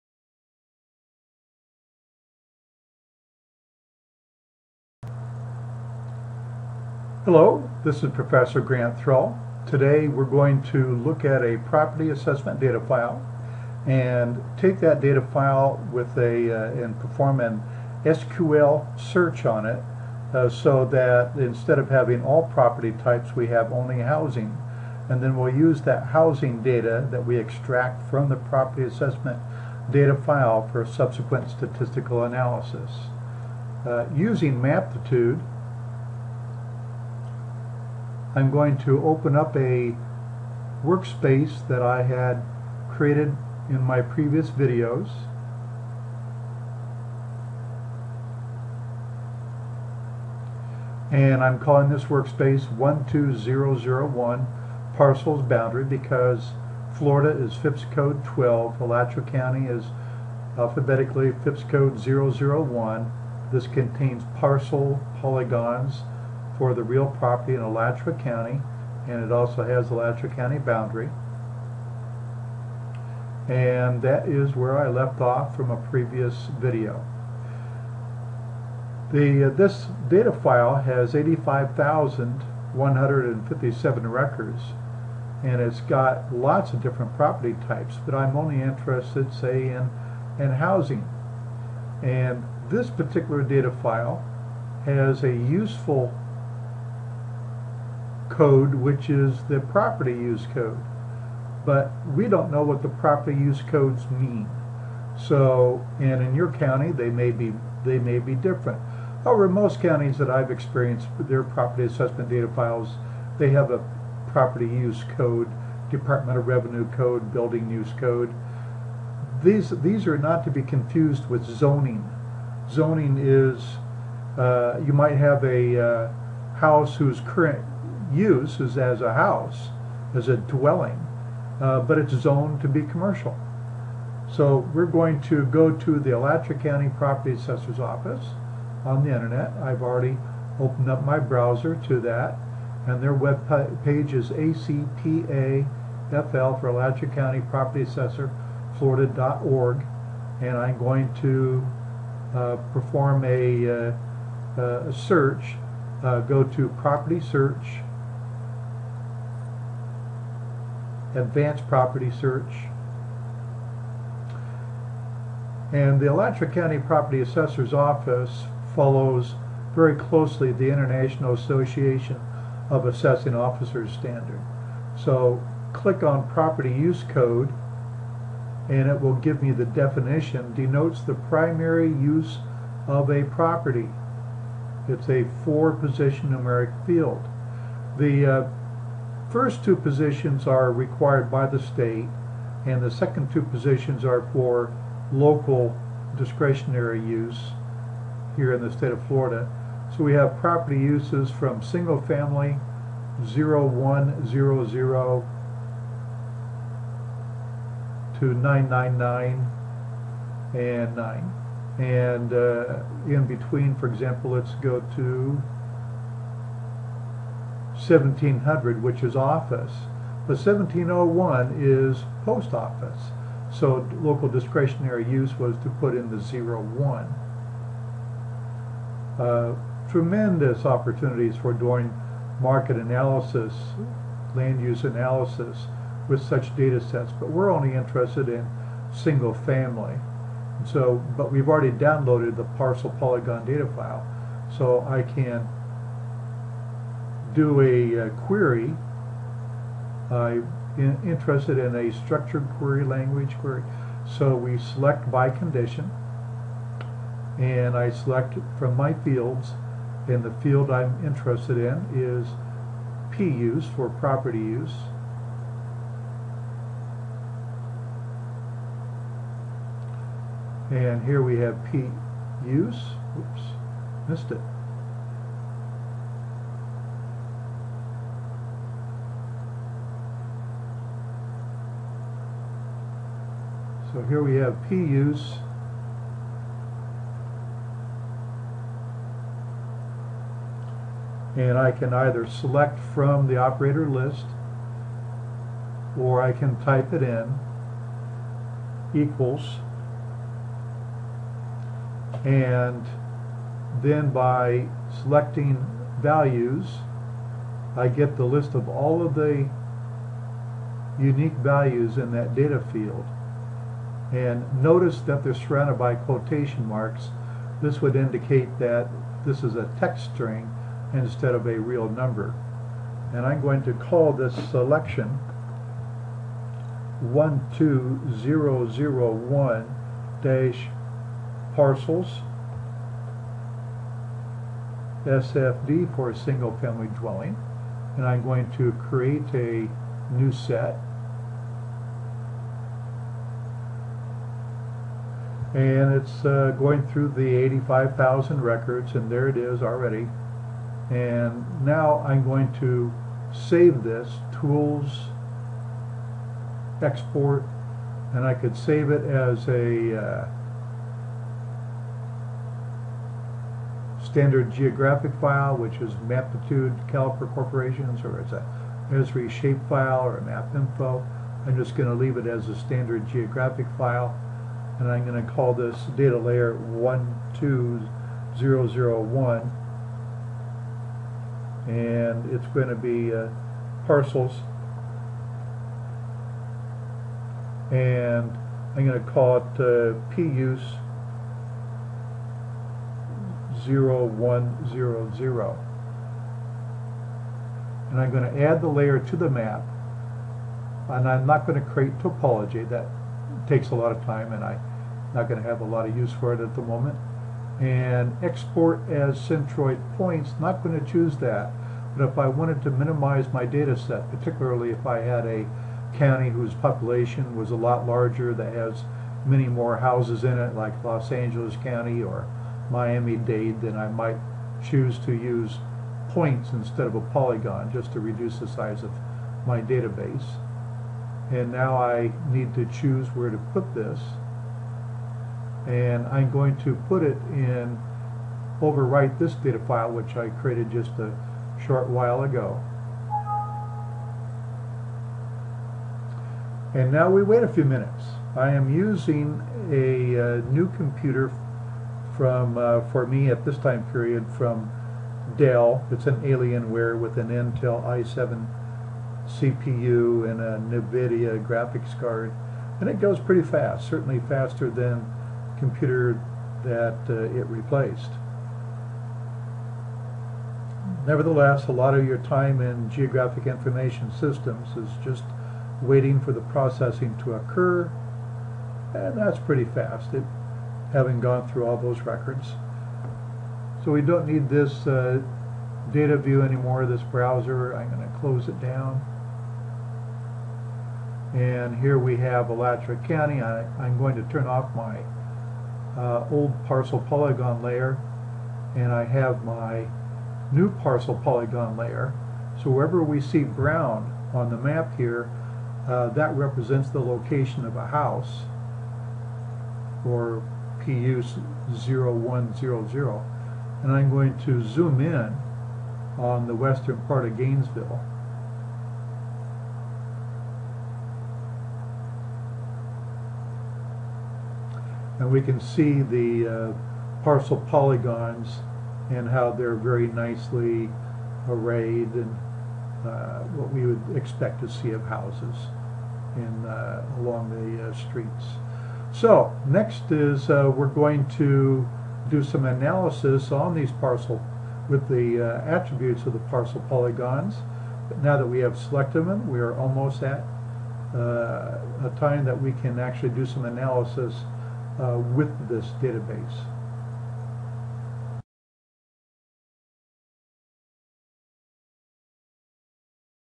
Demo 6i: SQL Conditional Selection In GIS To Create A Housing Database
SQL Conditional Selection In GIS To Create A Housing Database. Uses property polygons joined to a property assessor data file that included "property use" code PUSE. Example is from Gainesville Florida, in Alachua County.